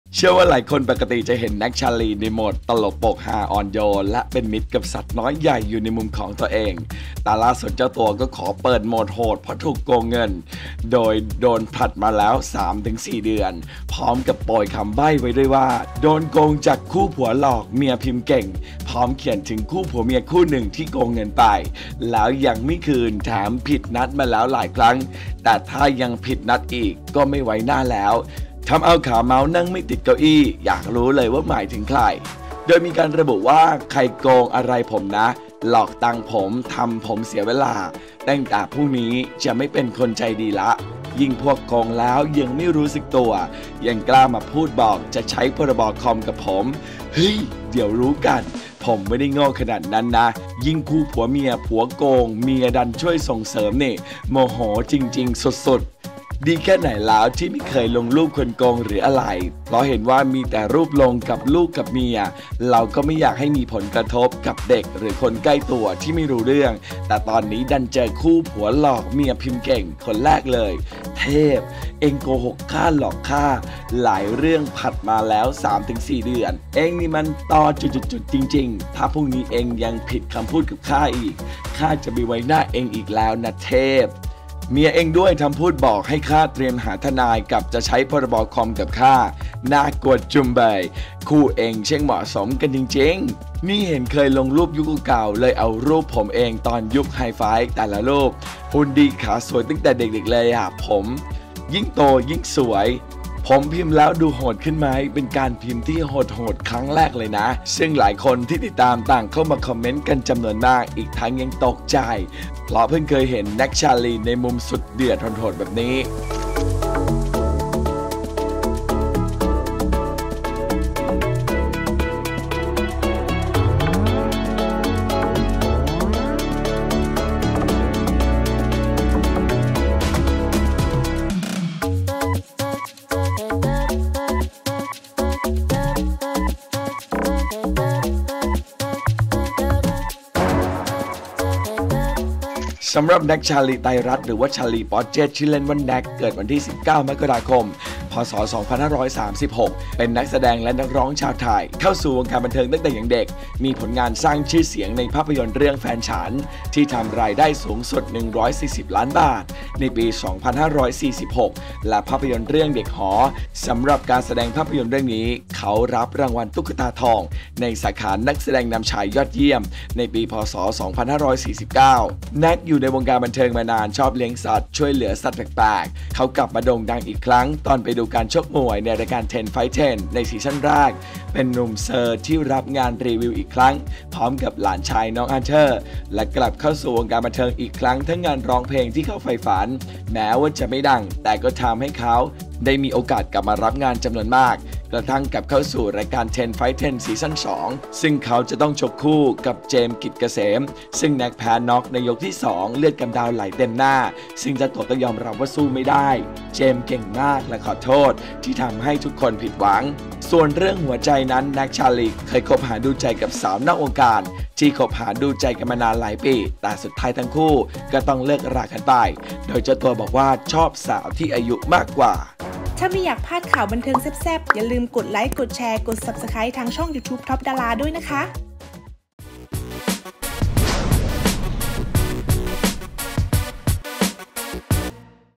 "แน็ก ชาลี" เดือดจัดด่าใคร "ผัวหลอกเมียพิมพ์เก่งโกงเงิน ถ้าผิดคำพูดไม่ไว้หน้าแล้ว" | TOP DARA
เชื่อว่าหลายคนปกติจะเห็น "แน็ก ชาลี" ในโหมดตลกโปกฮา อ่อนโยนและเป็นมิตรกับสัตว์น้อยใหญ่อยู่ในมุมของตัวเอง แต่ล่าสุดเจ้าตัวก็ขอเปิดโหมดโหด เพราะถูกโกงเงินโดยโดนผลัดมา 3-4 เดือนแล้ว พร้อมโปรยคำใบ้ไว้ด้วยว่า โดนโกงจากคู่ "ผัวหลอก เมียพิมพ์เก่ง" พร้อมเขียนถึงคู่ผัวเมียคู่หนึ่งที่โกงเงินไป แล้วยังไม่คืน แถมผิดนัดมาหลายครั้ง แต่ถ้ายังผิดนัดอีก ก็จะไม่ไว้หน้าแล้ว ทำเอาขาเมาท์นั่งไม่ติดเก้าอี้ อยากรู้เลยว่าหมายถึงใคร?!

ติดตามความเคลื่อนไหวจาก TOP DARA ได้อีกช่องทาง FB TOP DARA